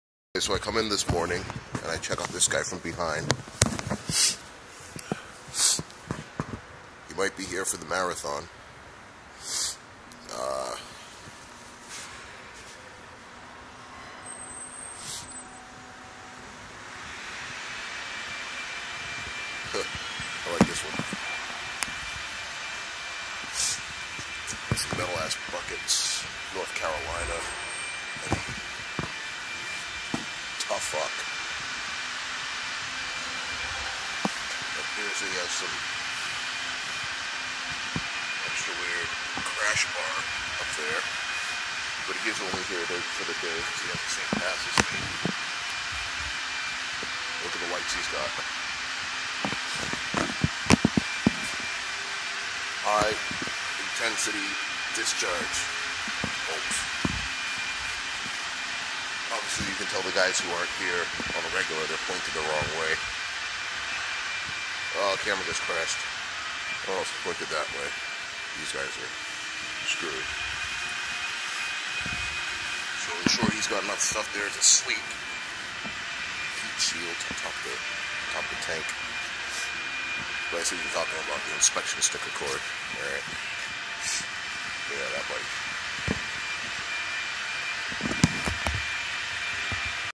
Drove in the marathon
Saw this stacked beast in the garage at work.